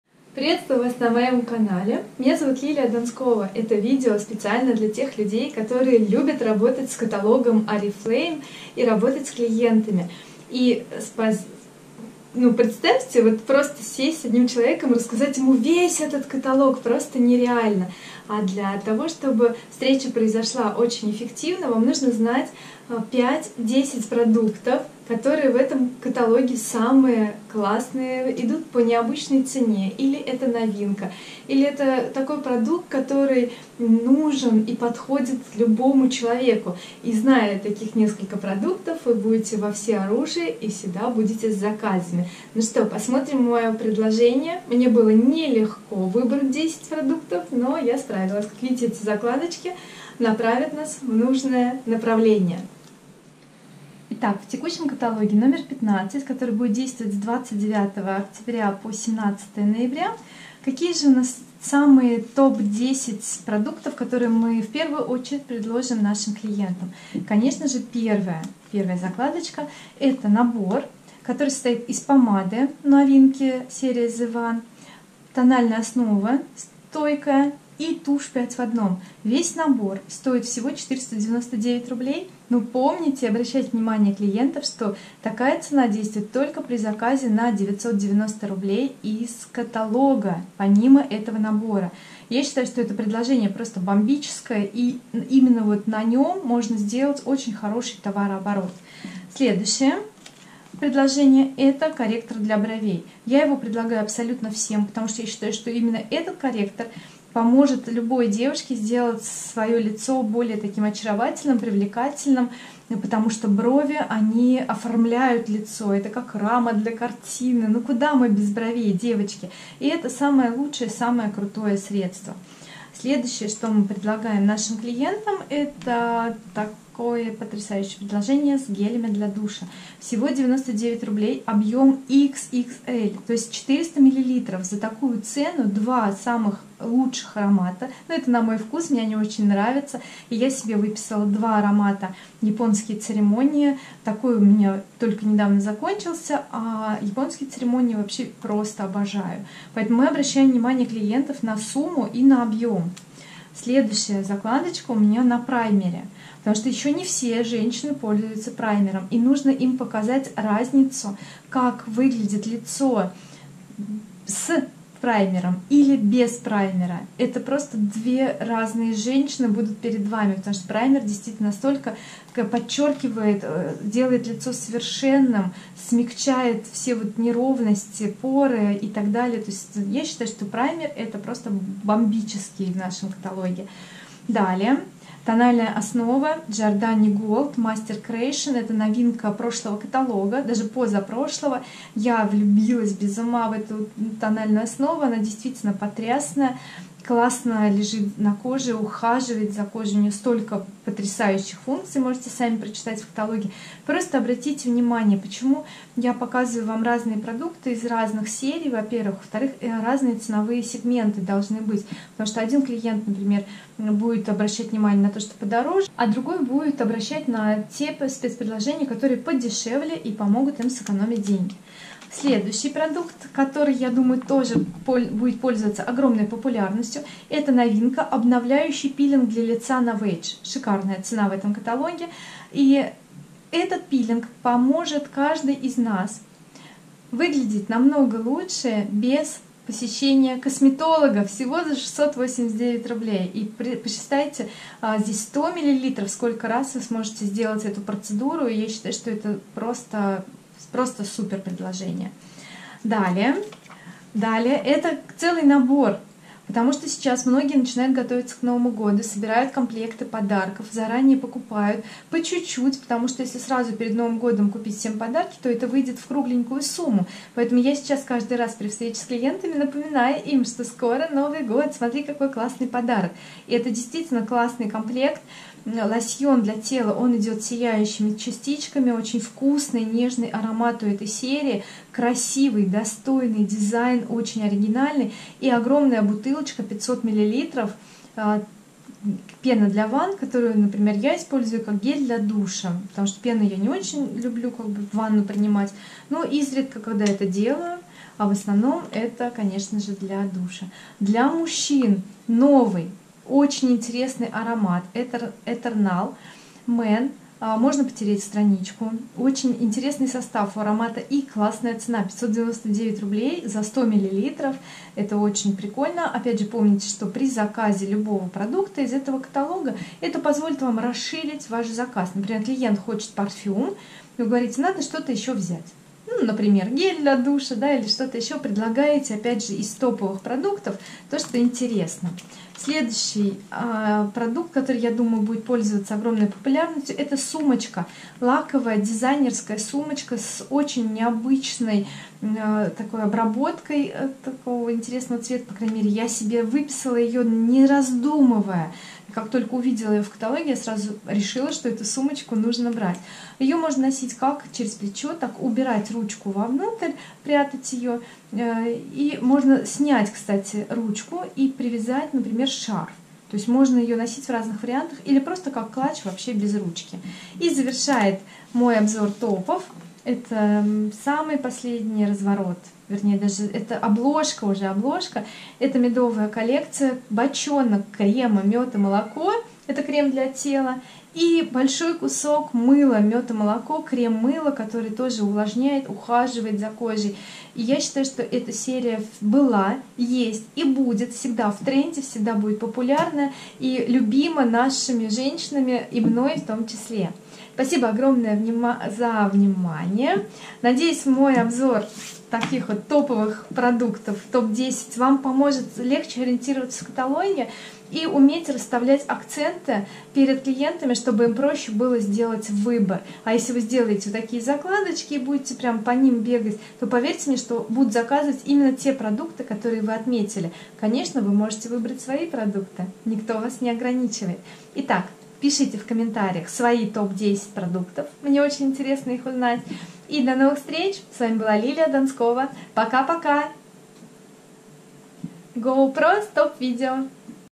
МОЙ ТОП-10 ПРОДУКТОВ в каталоге Oriflame №15-2018
Всегда при общении с клиентами будьте готовы акцентировать внимание на нескольких ключевых продуктах каталога. Для себя выделила ТОП-10 таких продуктов из текущего каталога Oriflame №15-2018!
А что выберете Вы?))